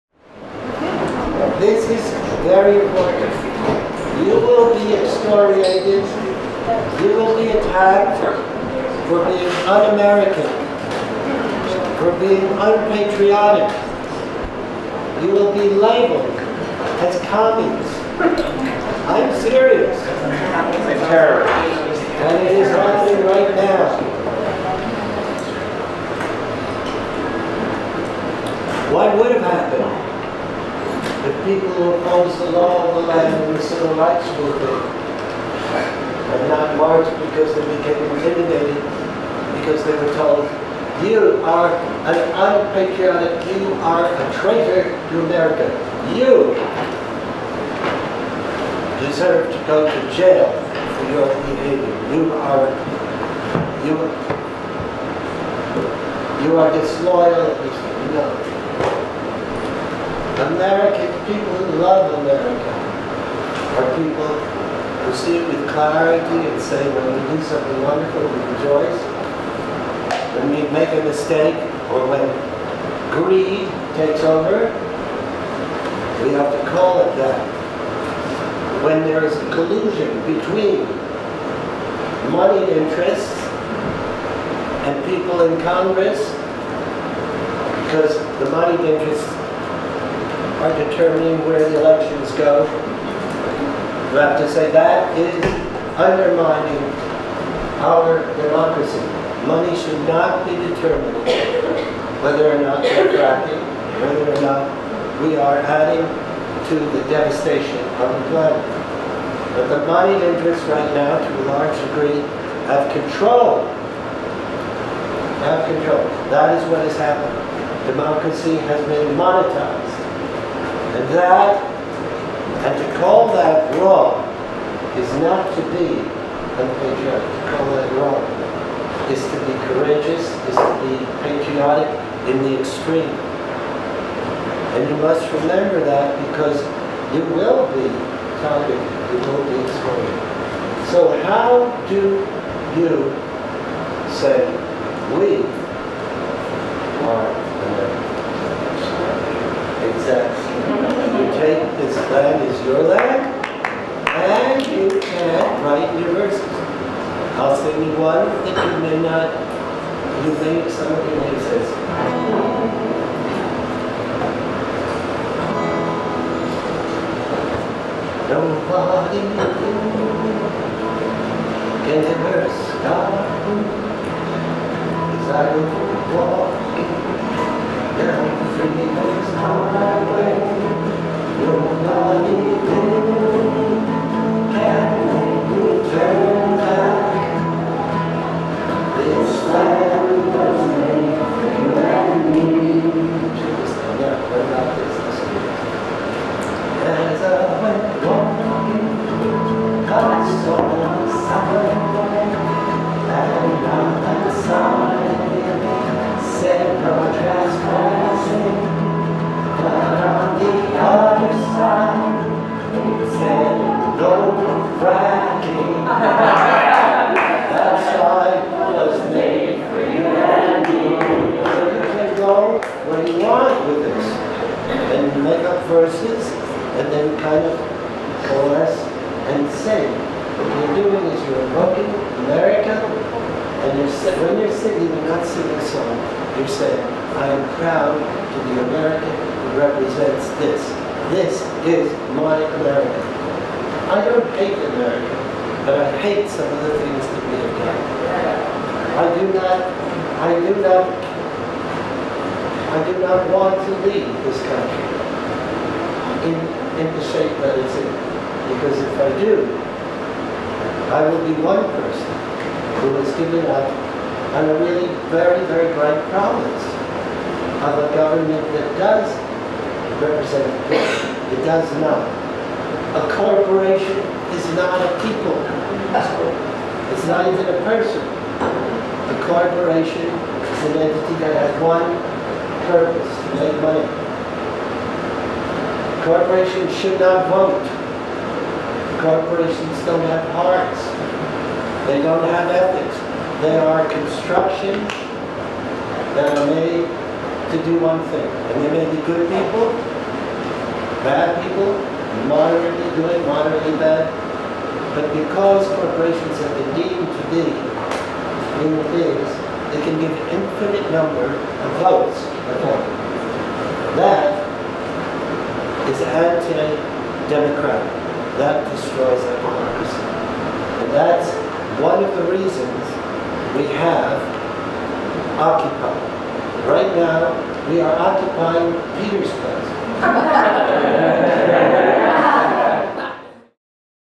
Yarrow Master Class Part 6a, Talk on monetization of our government, and true love of country
Peter spends a few minutes on the corruption of our democracy, leading to Occupy's response.  He also talks about the corruption of the soul that leads people to excoriate those who criticize our country's policies as disloyal.  True love of country involves acknowledging its faults and working towards the better.